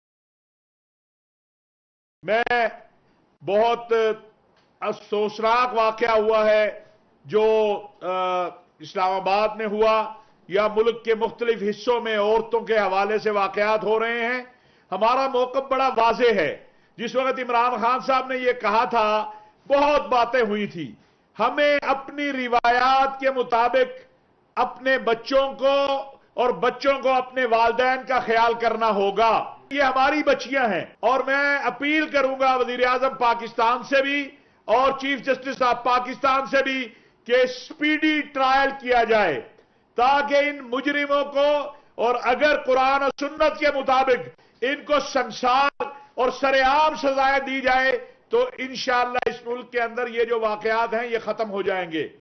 Ashrafi demands speedy trial of culprits involved in heinous crimes against women
ISLAMABAD, Jul 26 (APP):Special Representative to the Prime Minister on Religious Harmony and Middle East Hafiz Tahir Mehmood Ashrafi on Monday demanded to public hanging of the perpetrators of heinous crimes against women.
Addressing a news conference, Tahir Ashrafi, who is also chairman of Pakistan Ulema Council (PUC) appealed Chief Justice of Pakistan and Prime Minister to order speedy trial of Noor Muqaddam murder and Usman Mirza cases and stressed public hanging and stoning to death of the culprits.